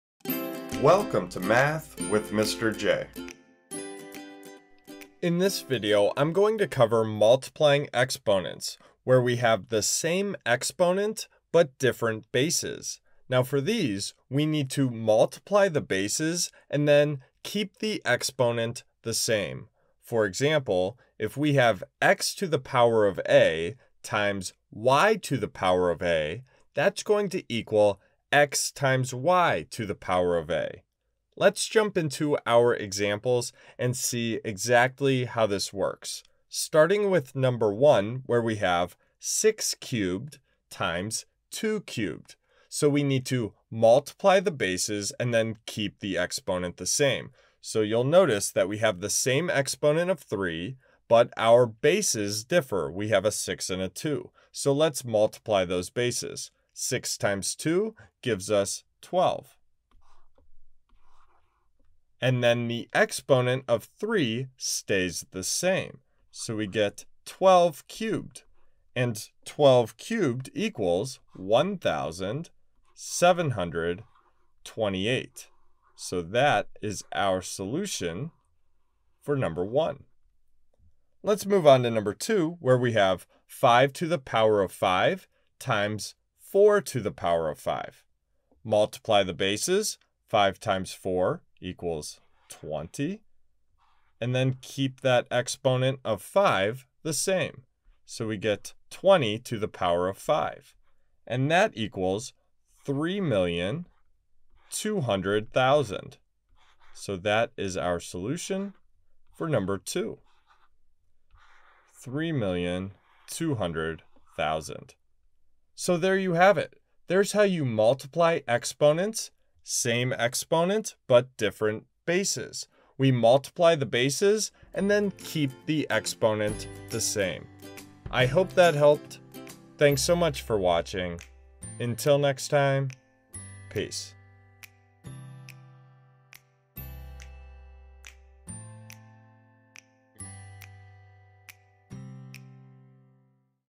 Multiplying Exponents with Different Bases and the Same Exponent | Math with Mr. J
Welcome to Multiplying Exponents with Different Bases and the Same Exponent with Mr. J! Need help with exponents (aka - powers)? You're in the right place!

Whether you're just starting out, or need a quick refresher, this is the video for you if you're looking for help with multiplying exponents. Mr. J will go through multiplying exponent examples and explain the steps of how to multiply exponents with different bases and the same exponent.

Hopefully this video is what you're looking for when it comes to multiplying exponents with the same exponent, different bases.